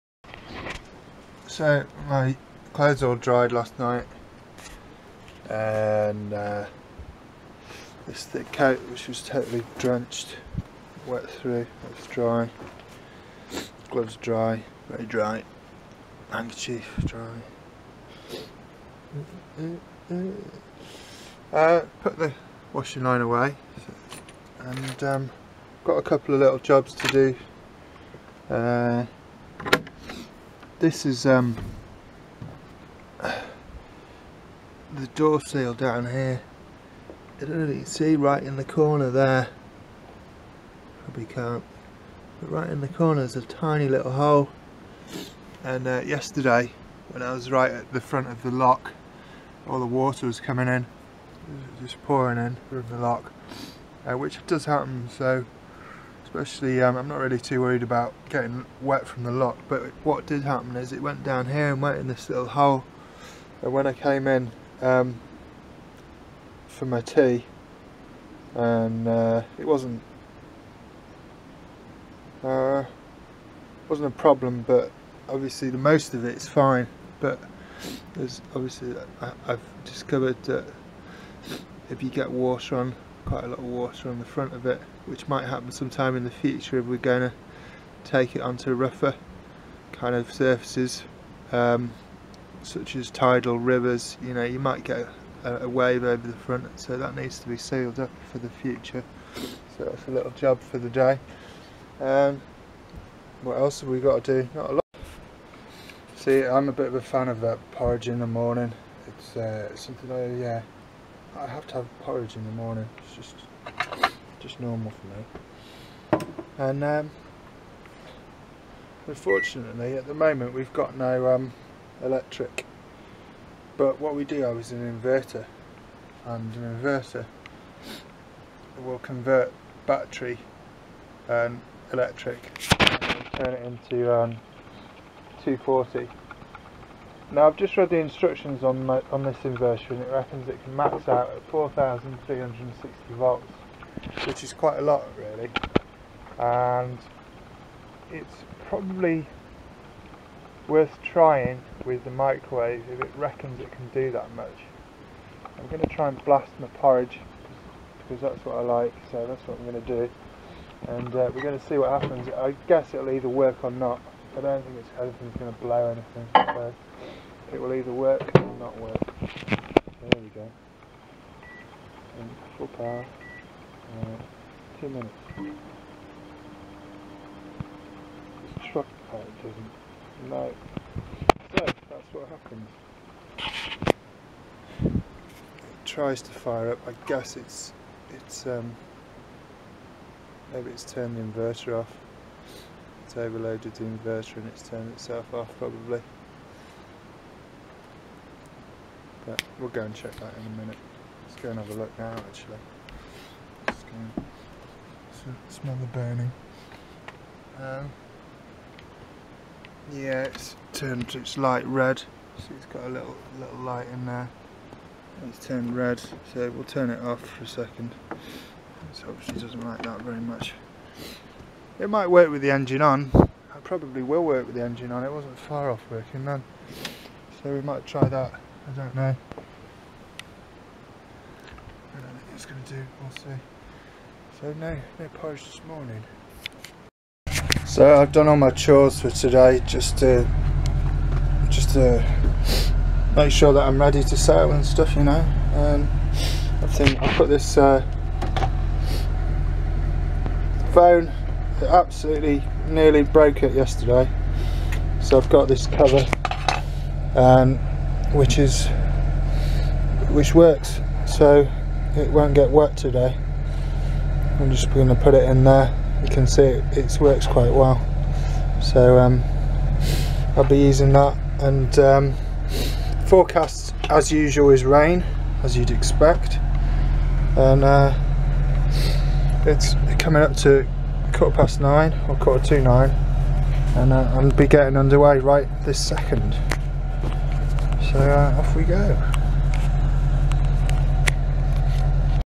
Bonny Journey - Day 2
My journey from John O'Groats to lands end, narrowboat phase.
Currently on day 2 of the narrowboat journey up to the northern edges of the UK canal system.
Today's journey takes me from Calfs Heath, Staffs to Gnosall Shrops.
In this video; how a microwave works with a 1000W Besteck inverter and leisure batteries.